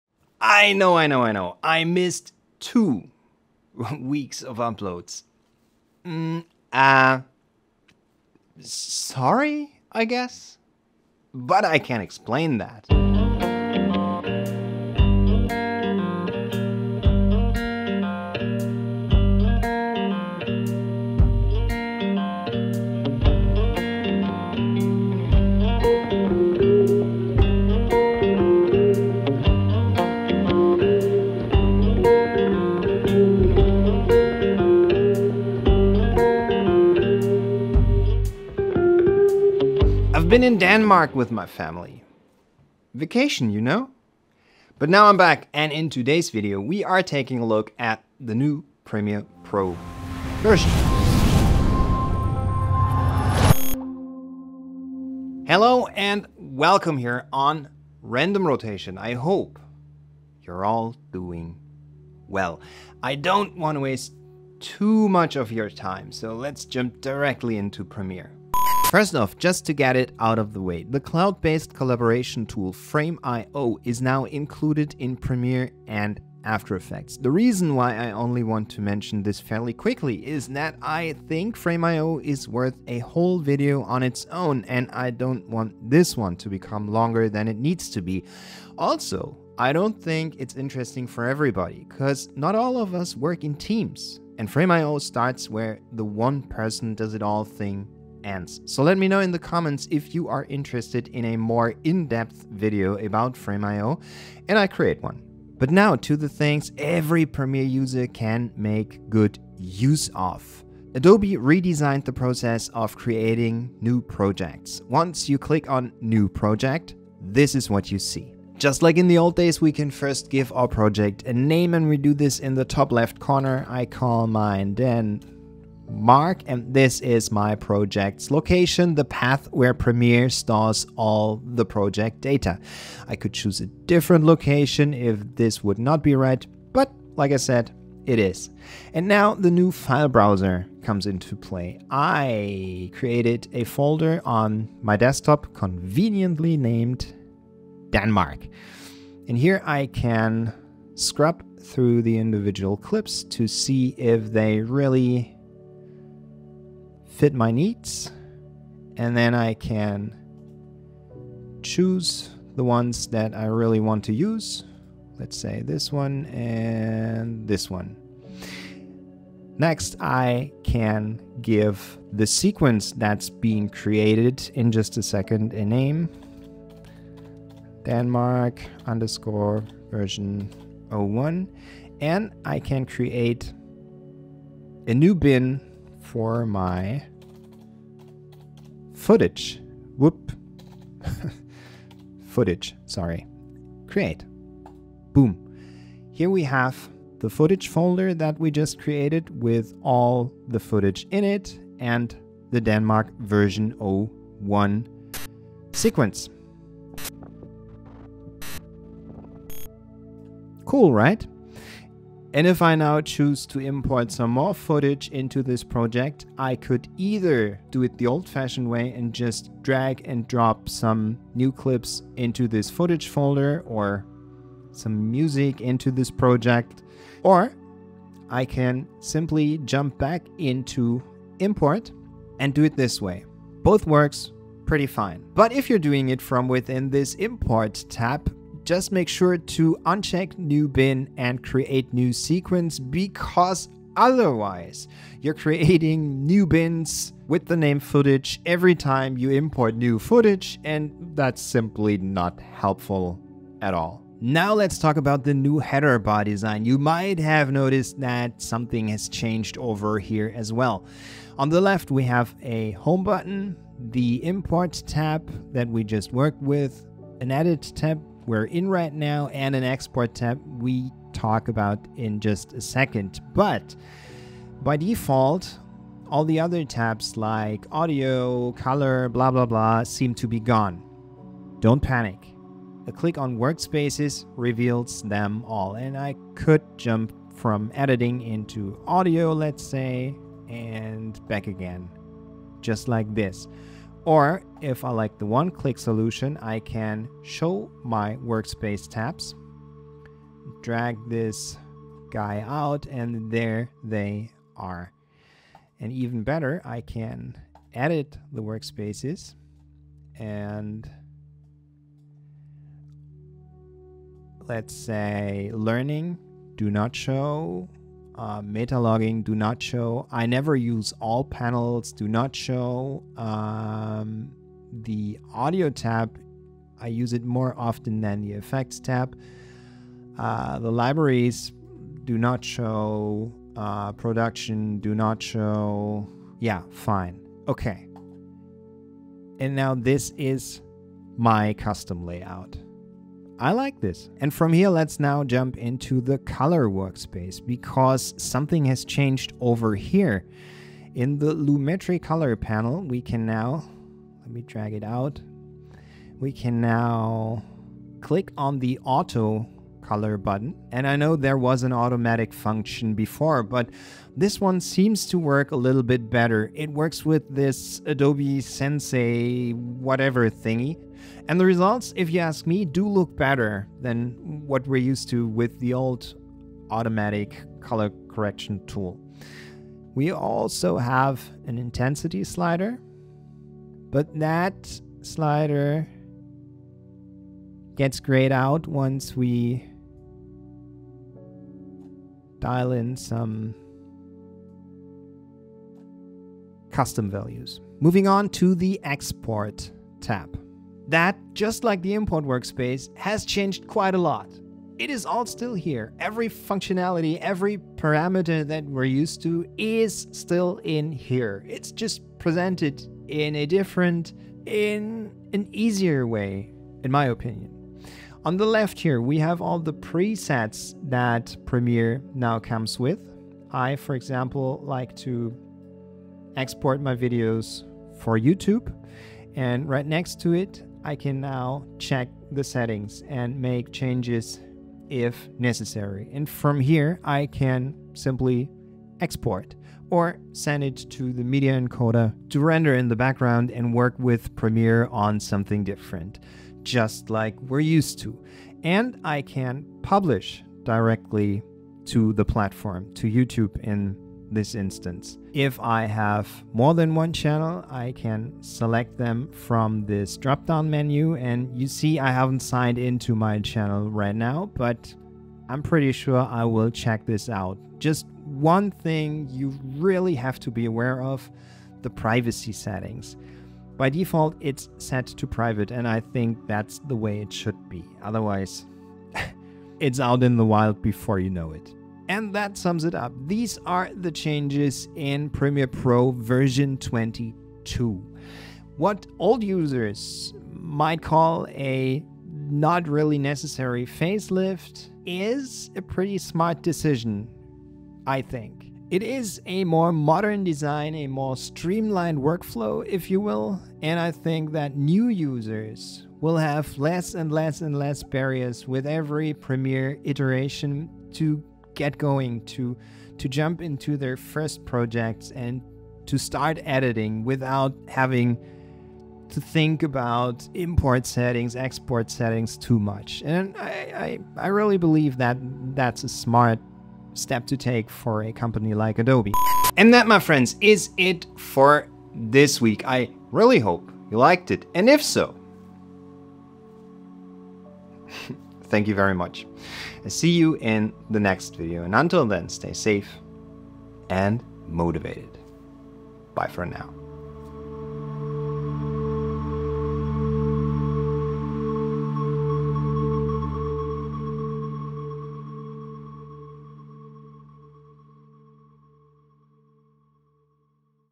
What's new in Premiere Pro V22.3?
There is a new version of Premiere Pro out. Adobe redesigned the Import and Export workspaces, integrated Frame IO, not only in Premiere, but also in AE. And that’s not all. But for that you gotta watch the video :)

0:00 - Intro
0:55 - Hello
1:08 - Frame IO
1:50 - Creating new projects
4:14 - New Header Bar
5:44 - Color workspace
6:37 - New export workspace
8:49 - Outro

Thanks everybody!!!